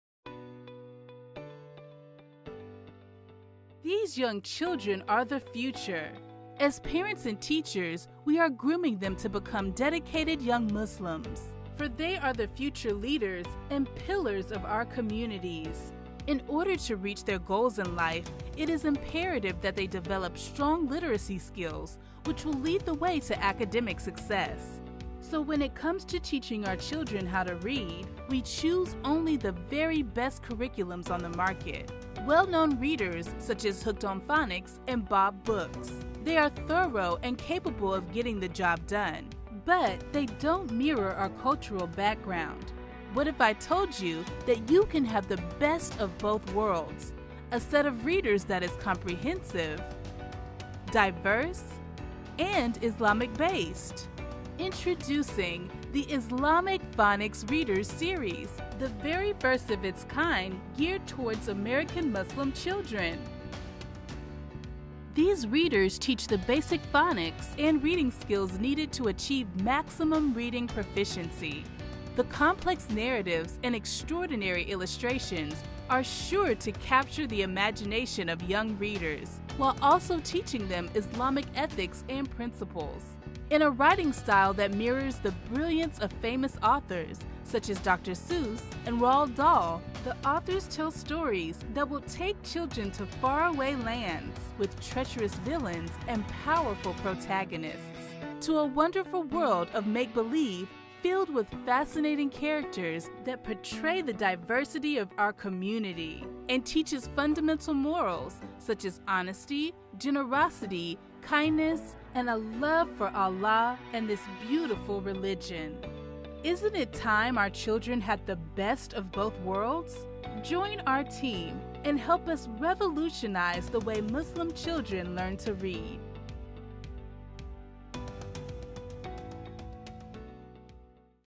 The Islamic Phonics Readers; Revolutionizing The Way Muslim Children Learn To Read
Are you looking for an Islamic, diverse, and engaging reading series for your young learner? We've got you covered! With the help of Allah (SWT), we are creating a high-quality reading curriculum that teaches the fundamentals of reading, while instilling a strong sense of Muslim identity and love for Islam in Muslim children.